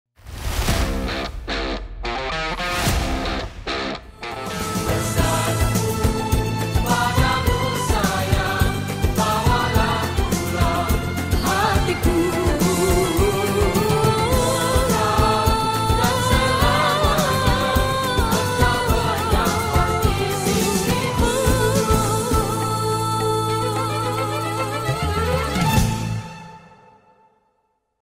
Muzikita Sri Mahligai June Teaser @ Persada Budaya
Catch Sri Mahligai's performance accompanied by Sriwana's dancers on 8 June at Persada Budaya only at WGS! 8pm to 9pm, free admission.😉

Saksikan persembahan Sri Mahligai ditemani penari-penari Sriwana pada 8 Jun di Persada Budaya hanya di WGS! 8 malam hingga 9 malam, kemasukan percuma.😉